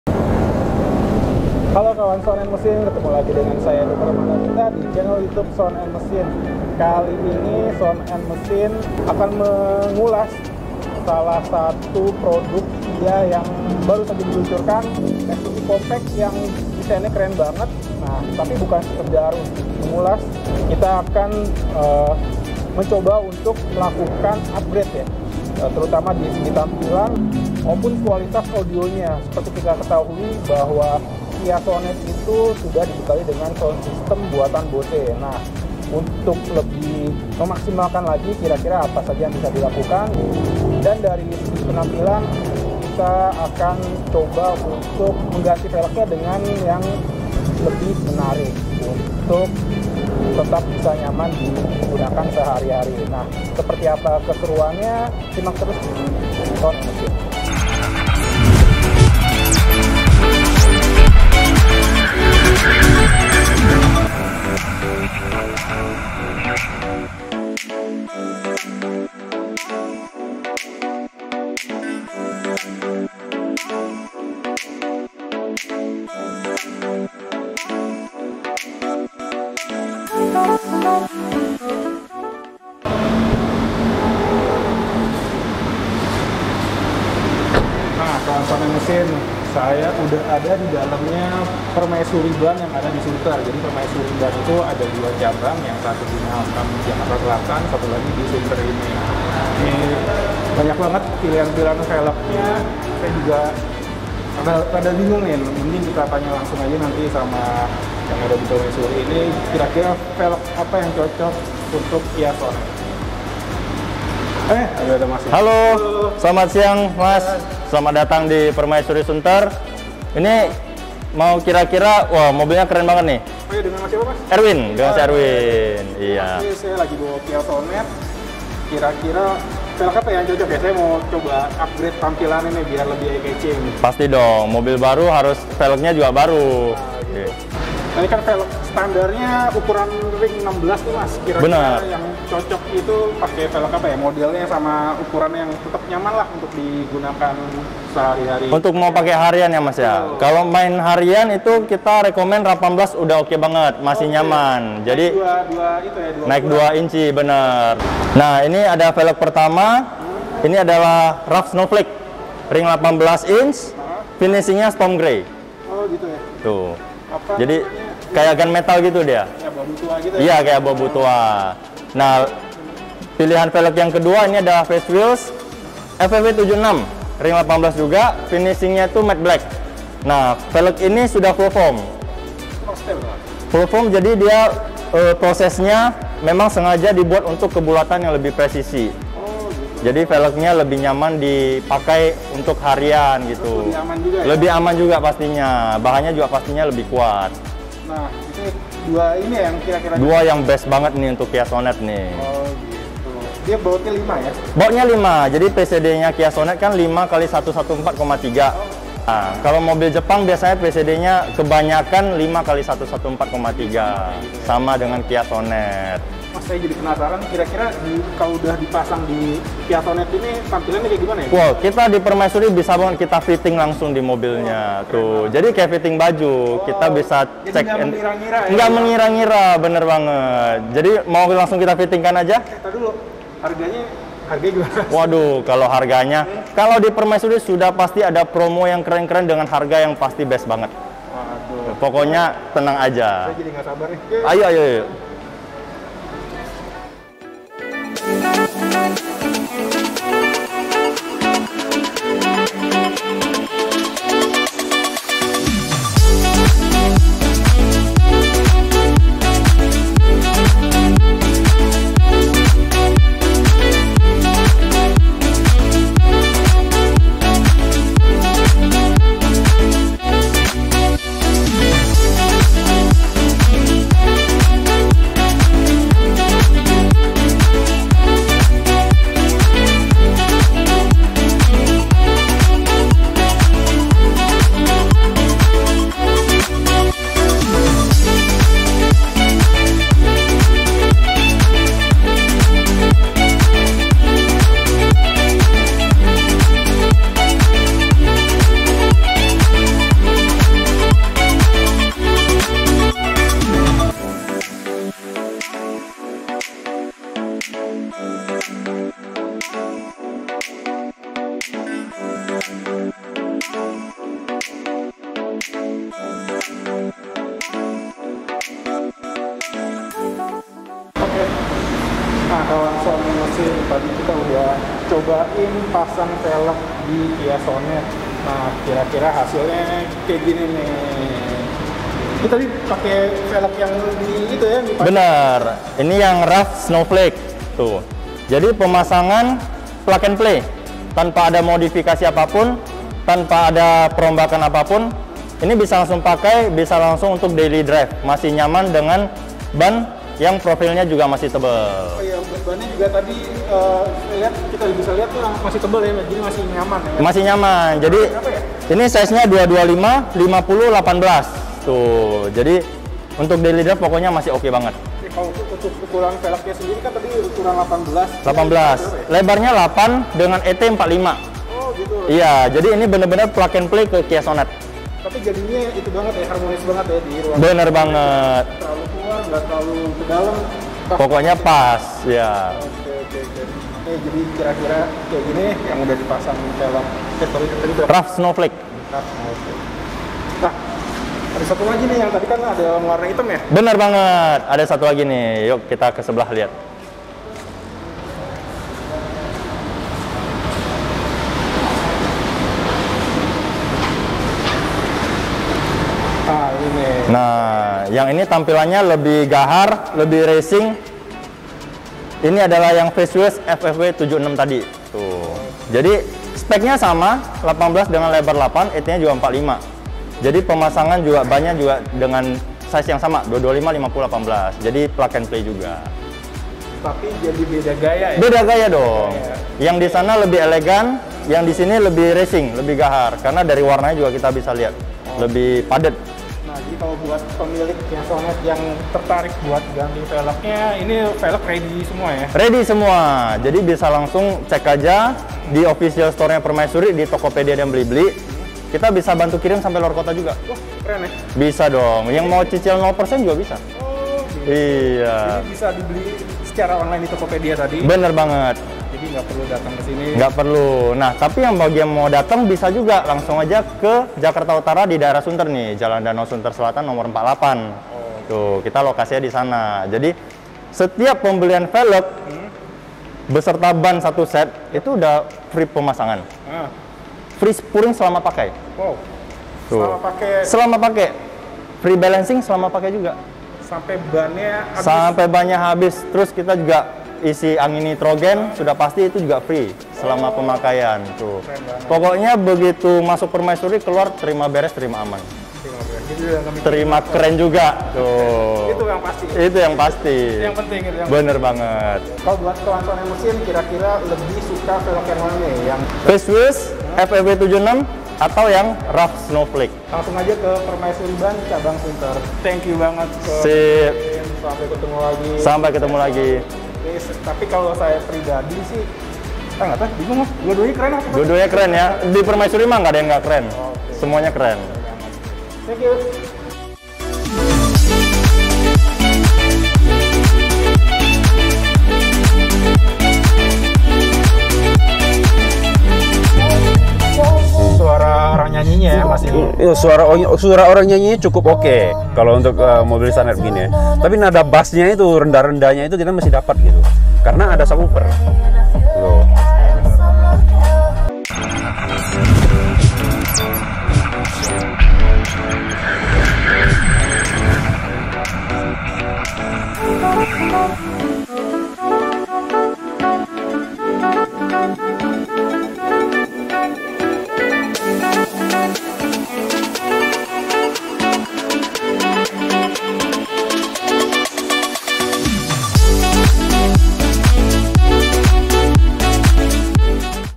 UPGRADE [Part 1] : KIA SONET 1.5L Premier - Ganti Pelek di Permaisuri Ban
Permaisuri Ban

Jl. Danau Sunter Selatan No.48, RT./RW:/RW.11/11, Sunter Jaya, Tj. Priok, Kota Jkt Utara, Daerah Khusus Ibukota Jakarta 14350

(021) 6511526